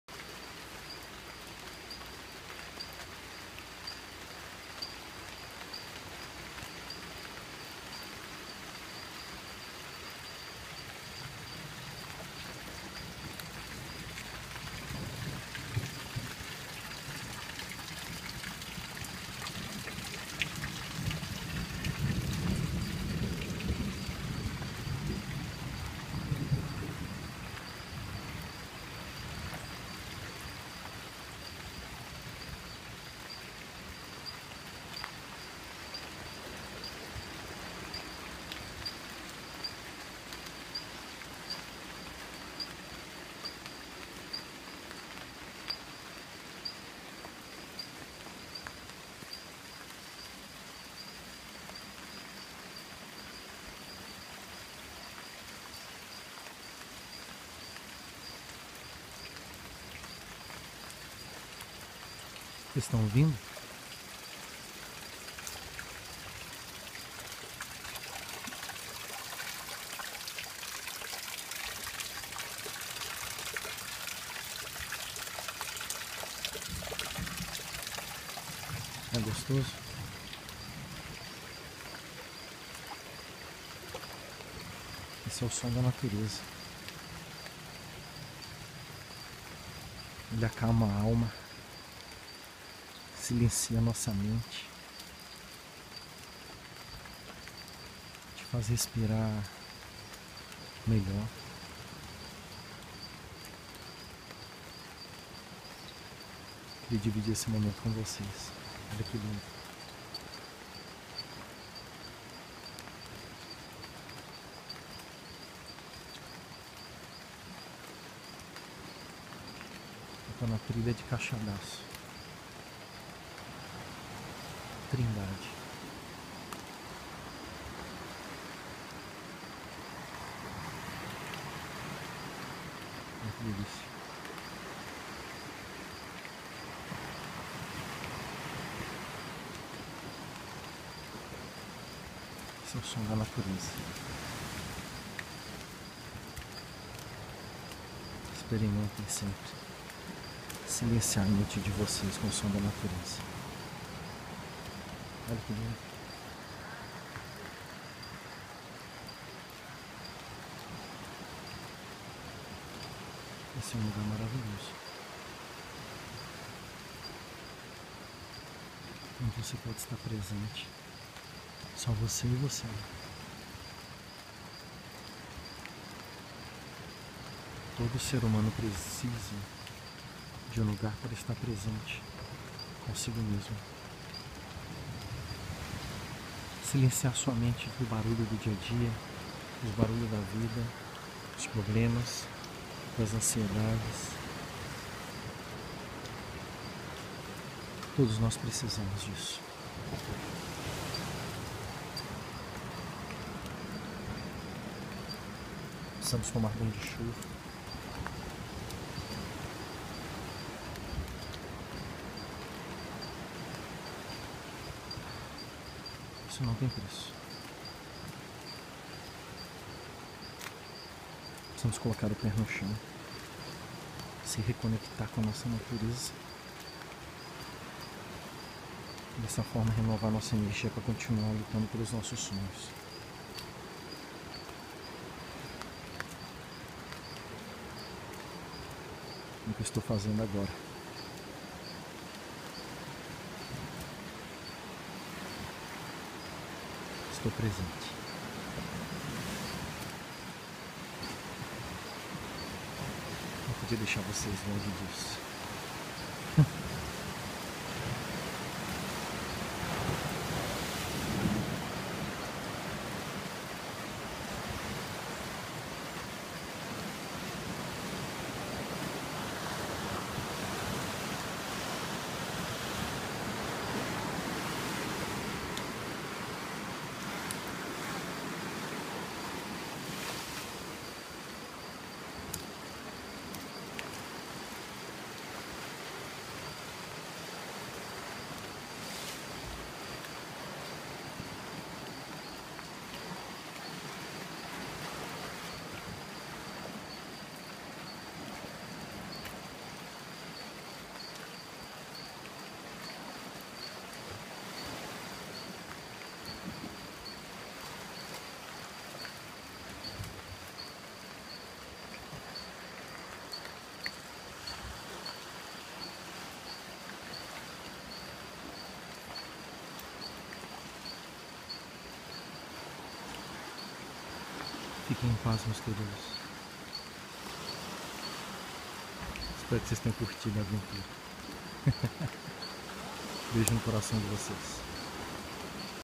Ouça e sinta a natureza!
Em uma das minhas aventuras dos últimos dias tive a oportunidade ímpar de desbravar a trilha da praia de Caixa D'aço em Trindade, no Rio de Janeiro. Estava chovendo enquanto caminhávamos... o som dos pássaros e das ondas misturados ao das árvores se traduzia em um mergulho dentro de mim mesmo em busca da minha essência e da paz que me acalma. Sugiro que fechem os olhos e ouçam os sons da natureza que reproduzo nesse divã...